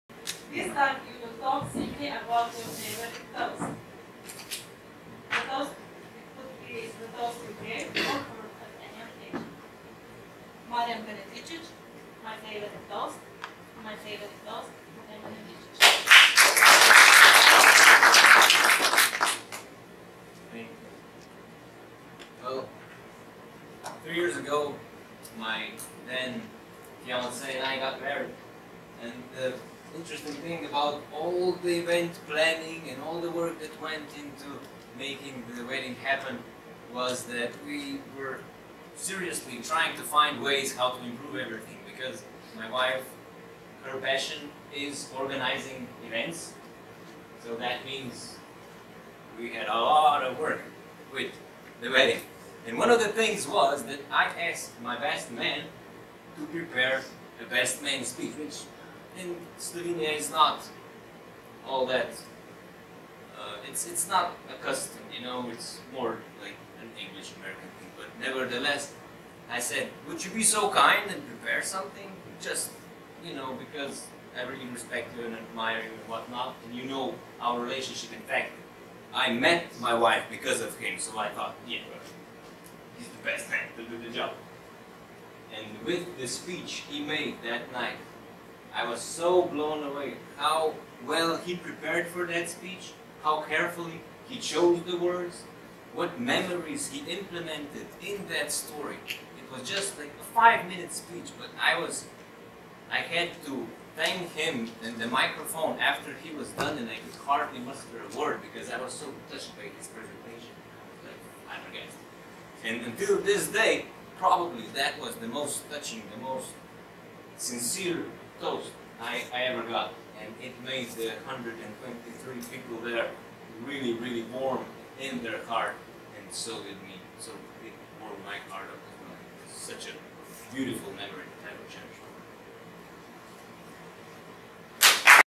Table Topics Competition Finale on 10. 11. 2018 - Marjan Benedičič
TT finale on Autumm Speaking Marathon organized by TM Ljubljana.

Contestant Marjan Benedičičis from TM Beograd.